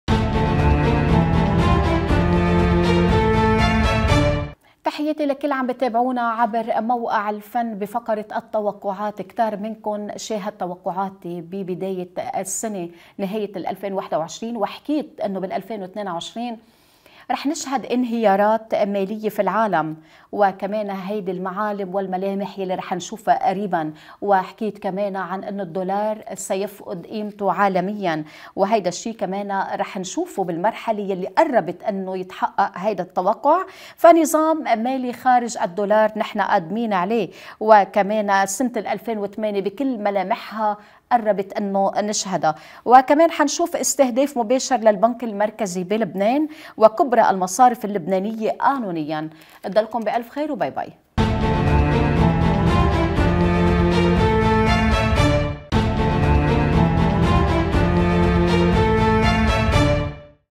جومانا وهبي :القضاء يحاسب المصرف المركزي ومصارف كبيرة في لبنان
تطل عالمة الفلك جومانا وهبي، أسبوعياً وعبر موقع "الفن"، في فقرة توقعات لشخصيات مشهورة من لبنان والعالم، ولأحزاب ودول.
في هذه الحلقة، تحدثت جومانا وهبي عن محاسبة قضائية تطال المصرف المركزي ومصارف كبيرة في لبنان، فما الذي كشفته؟
لمعرفة كل التفاصيل، تابعوا توقعات جومانا وهبي بالفيديو

Copyright : ElFann.com